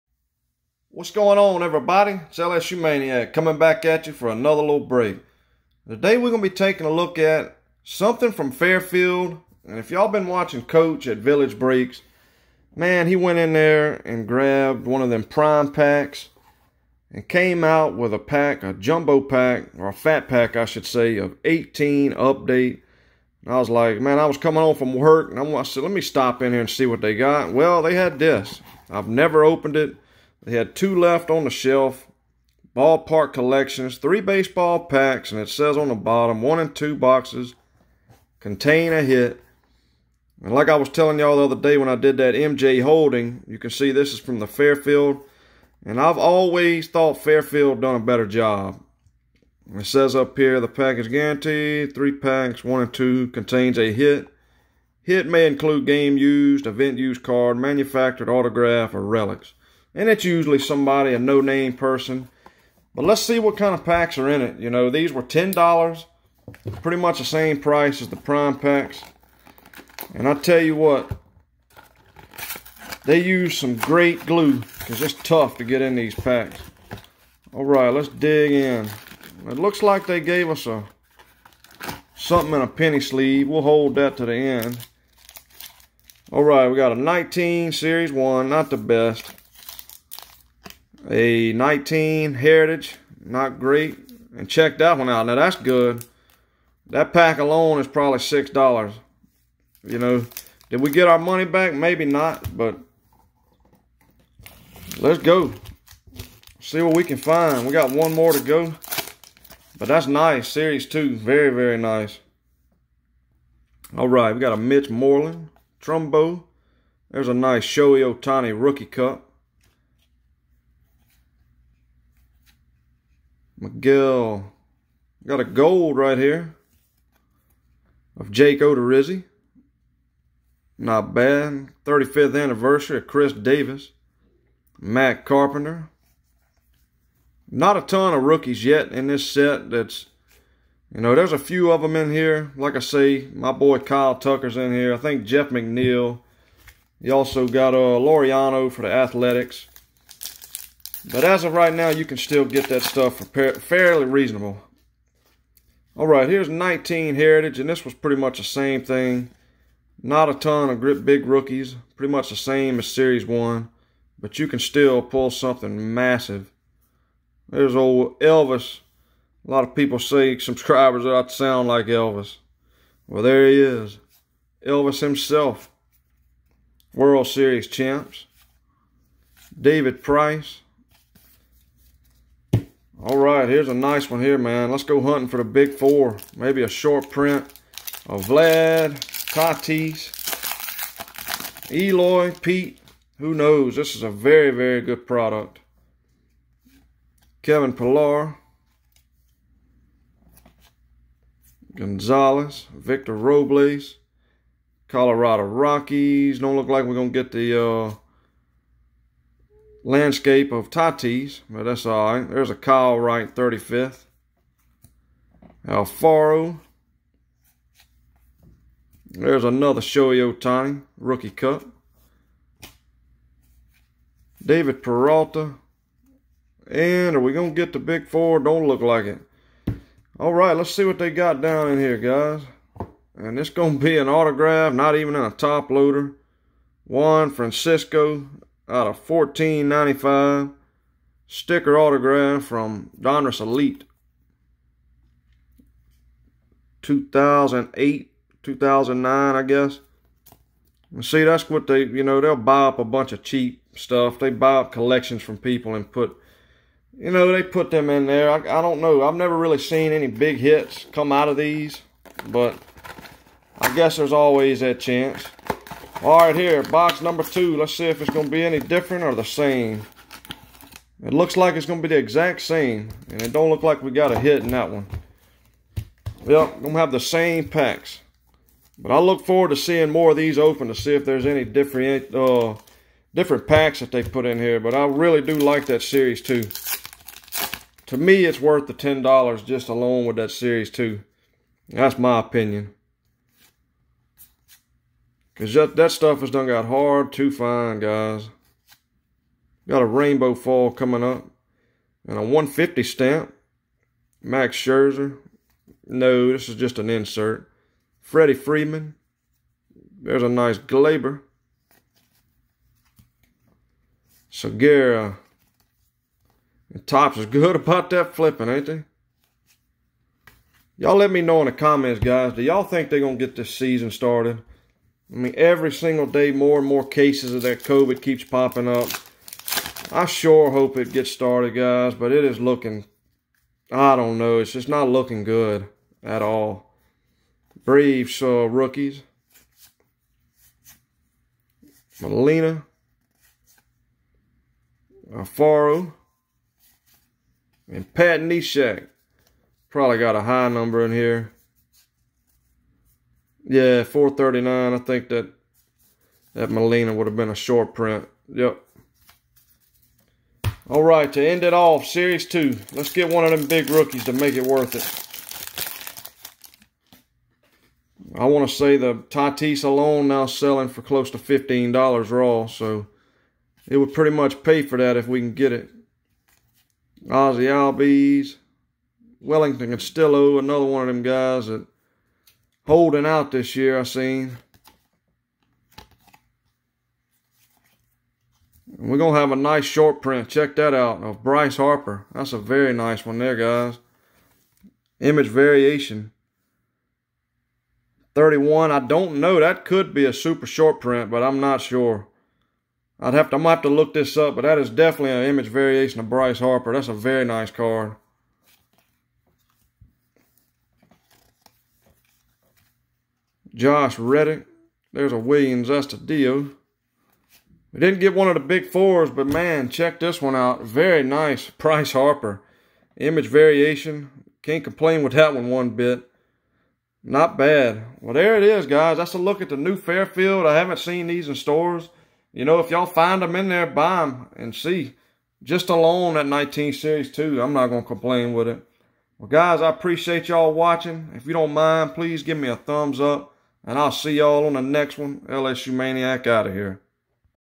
New Fairfield Mystery Box Ballpark Collection With 3 Packs.
In todays video we are looking at the new Fairfield box that you will find in your local pharmacy. They will run about $10 and contain 3 packs with 1 in 2 containing a hit. Let me know what you guys think of this product.